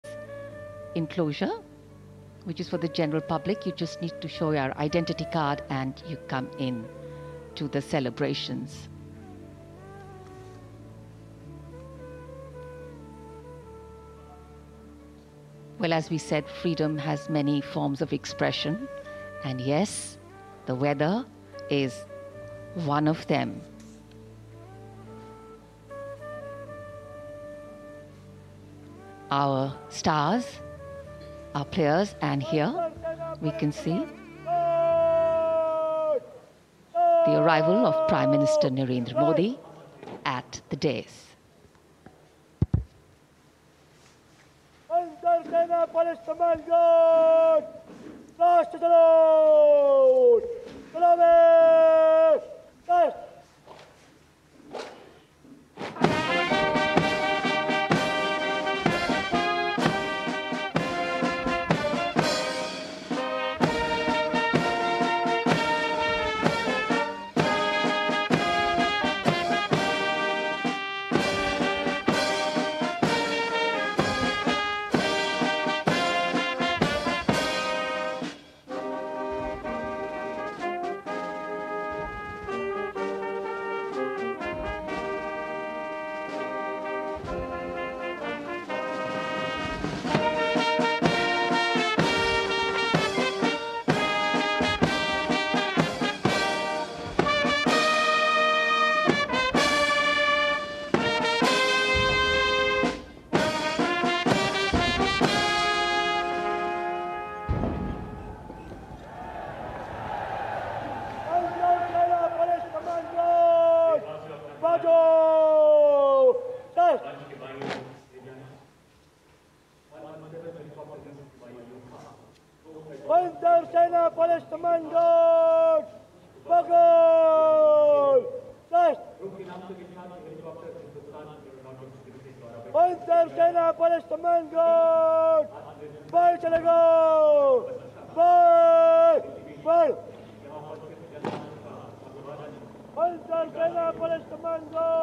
78th Independence Day | PM Modi ने लाल क़िले की प्राचीर पर फहराया तिरंगा
78th Independence Day | PM Modi ने लाल क़िले की प्राचीर पर फहराया तिरंगा